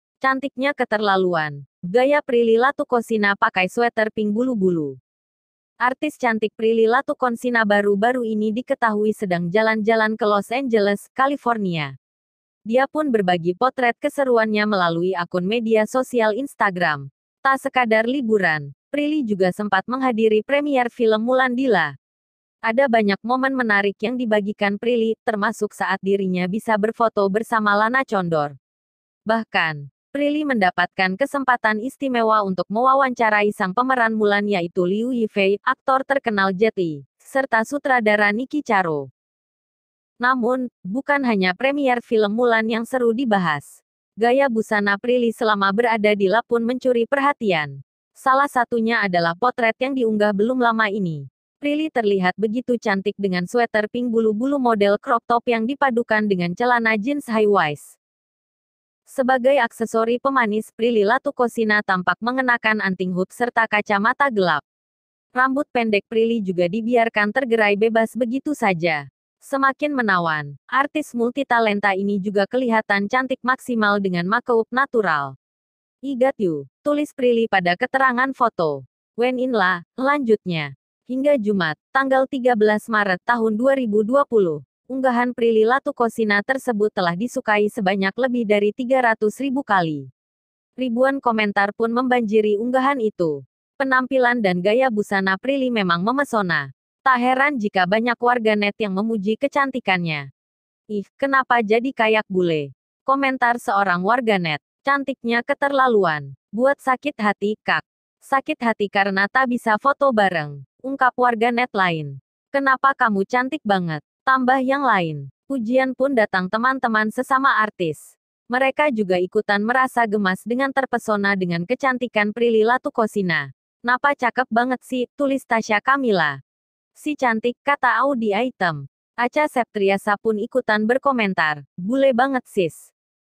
CANTIKNYA KETERLALUAN PRILLY LATUCONSINA PAKAI SWEATER PINK BULU- BULU
Artis cantik Prilly Latuconsinabaru-baru ini diketahui sedang jalan-jalan ke Los Angeles, California.
Tak sekadar liburan, Prilly juga sempat menghadiri premier film Mulan di LA. Ada banyak momen menarik yang dibagikan Prilly, termasuk saat dirinya bisa berfoto bersama Lana Condor.
Bahkan, Prilly mendapatkan kesempatan istimewa untuk mewawancarai sang pemeran Mulan yaitu Liu Yifei, aktor terkenal Jet Li, serta sutradara Niki Caro.